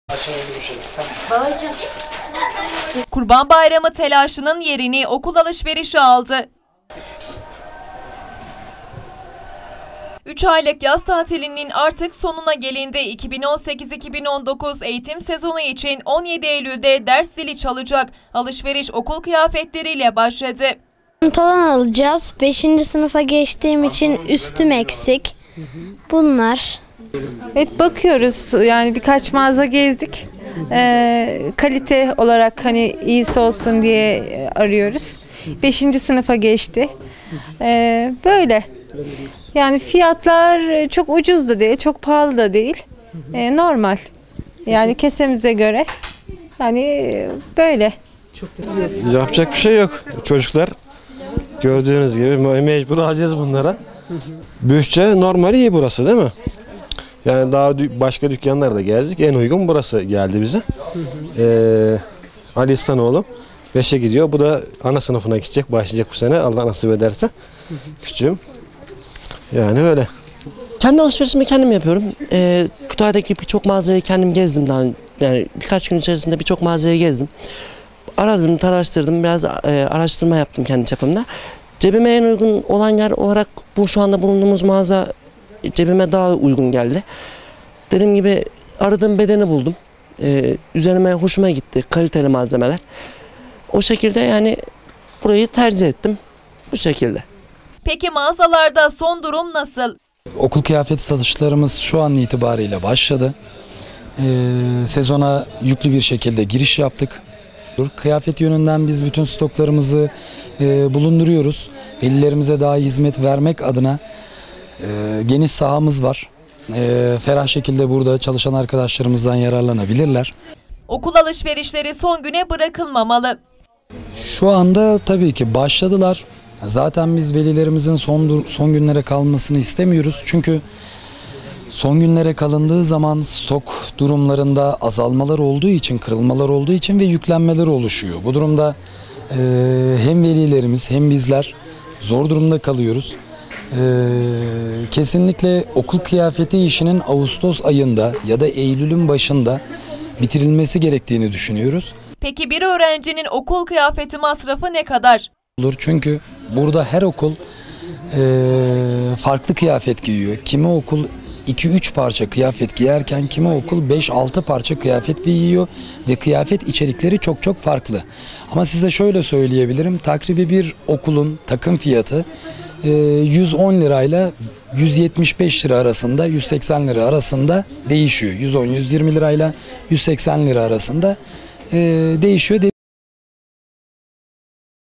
BAYRAM TELAŞI BİTTİ SIRA OKUL ALIŞVERİŞİNDE
Haber Metni :

BAYRAM TELAŞI BİTTİ, AİLELERİN ŞİMDİ DE OKUL TELAŞI BAŞLADI. UZUN BİR TATİL DÖNEMİNİN ARDINDAN ÖĞRENCİLER OKULLARINA VE ÖĞRETMENLERİNE KAVUŞMAYA HAZIRLANIRKEN, AİLELER DE OKUL İHTİYAÇLARI İÇİN ALIŞVERİŞE BAŞLADI.